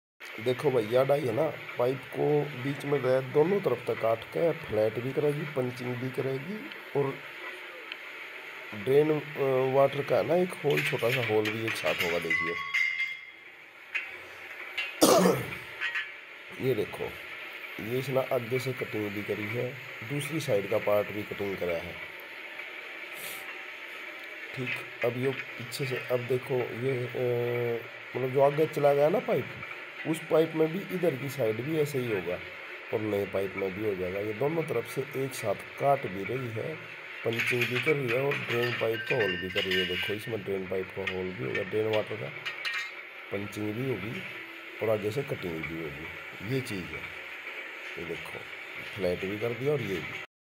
round pipe cutting tool in one strock both side
pipe ko donon taraf se flat kaise karen round pipe cutting press tool, round pipe cutting tool